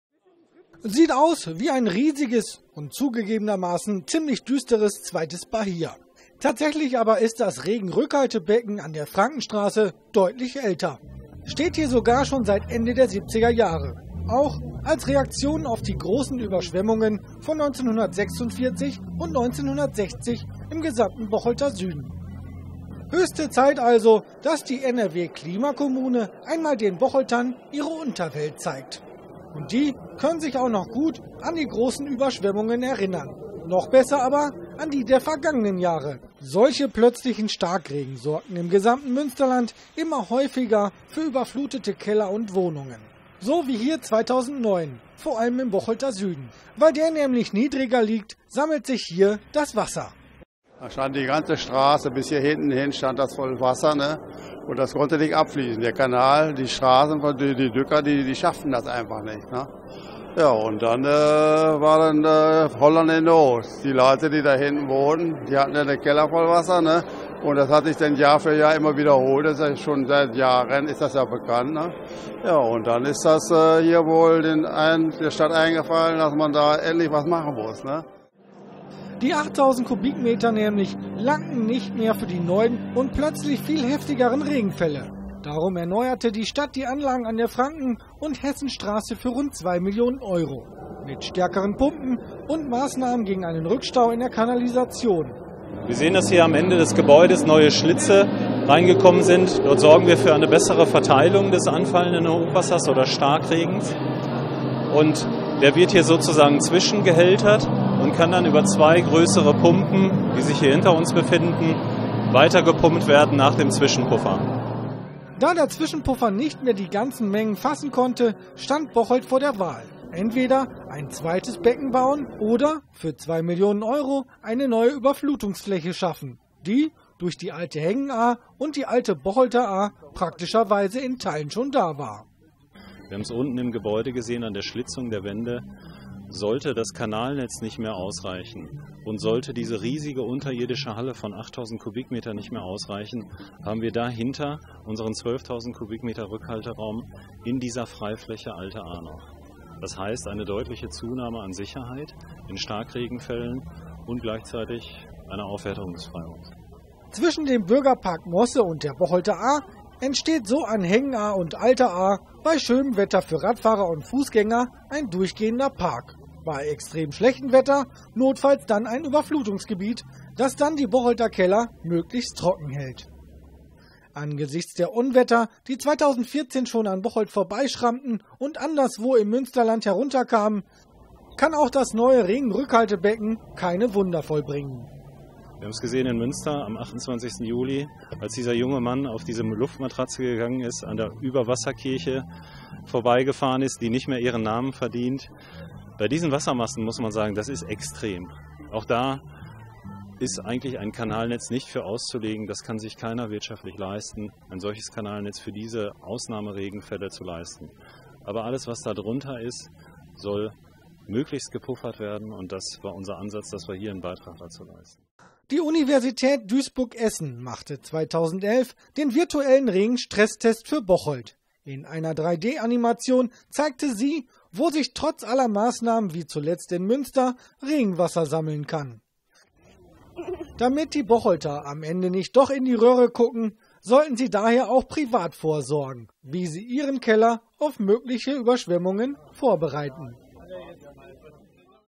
Bocholter Unterwelt hält den Regen zurück
Seit Ende der 70er hält das Auffangbecken an der Frankenstraße schon den Regen aus Bocholts Kellern. Nur weiß das kaum jemand und zudem gelang das in den vergangenen Jahren nicht immer. Mit Blick auf die immer häufigeren Starkregen erweitere die Klimakommune Bocholt jetzt das Regenrückhaltebecken - um einen Park.